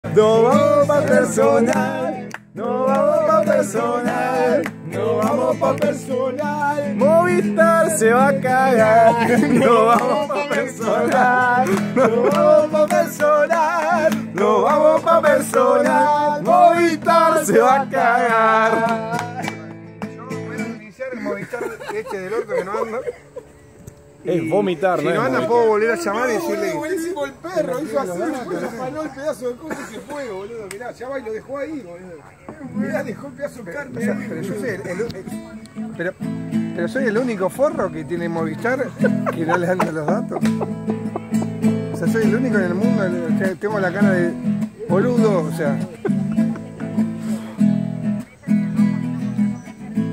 NOS VAMOS PA PERSONAL
Ariel Ra en un dia de pescadores y Asado !
En El Tigre Movistar no le daba Datos mopviles cuando el paga 500$ por mes en un super plan Full full !!!
Los muchachos de MAIORELL Band junto con Diego Palmeira crean un hit ! Que años mas tarde los convertiria en Ricos y famosos !